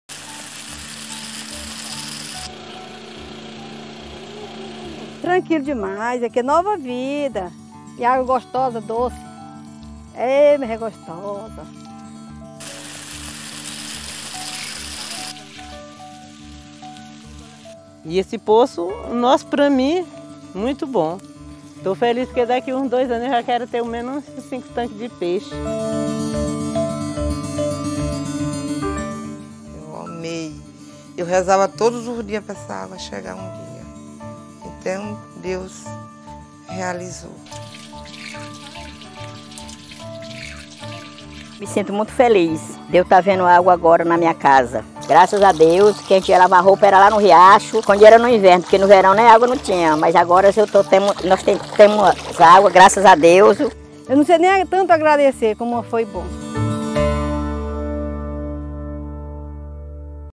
Governo comemora Dia Mundial da Água com ações por todo o Maranhão
O GovernoDoMaranhão desenvolve projetos para garantir água de qualidade e combater a pobreza no campo. Por meio dos programas Cisternas e ÁguaDoce, são distribuídos kits de irrigação e construídos poços artesianos.